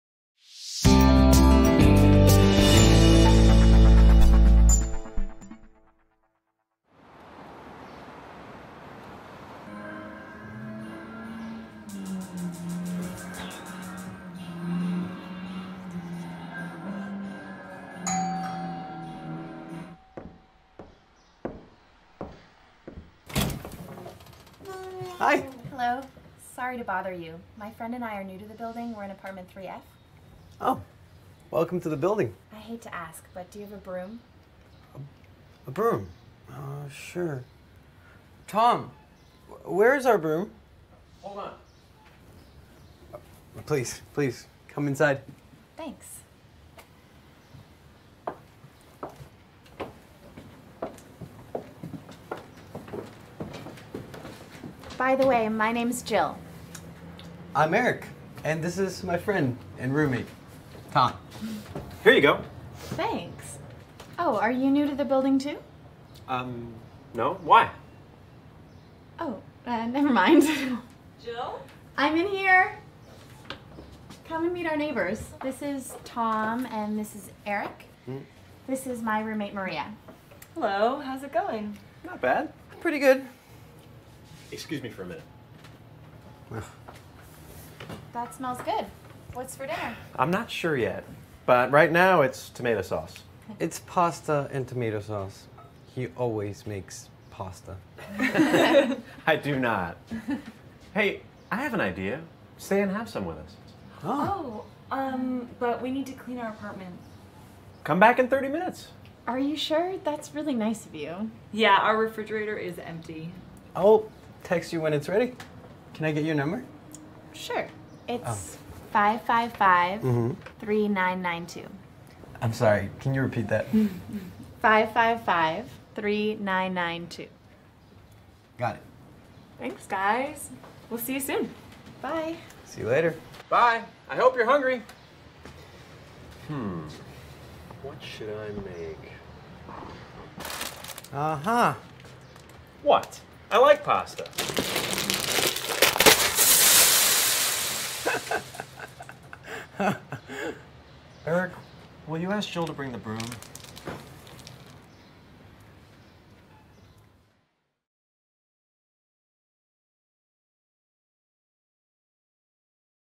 |English conversations | - SPEAK NOW 1 (Lesson 1- 4:New neighbors ）
Learning natural English from the movie/ videos with English and Vietnamese subtitles.

Speak now 1 (Lesson 1- 4:　New neighbors ）

Let's enjoy the video and learn some English.